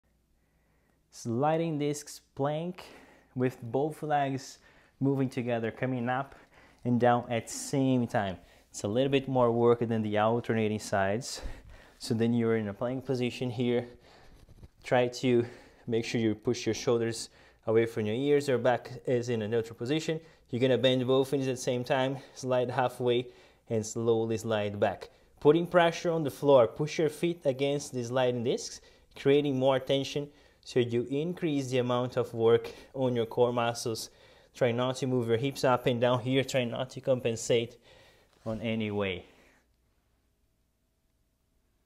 High Plank Sliding Discs - both legs at same time
Learn how to challenge your core with this exercise.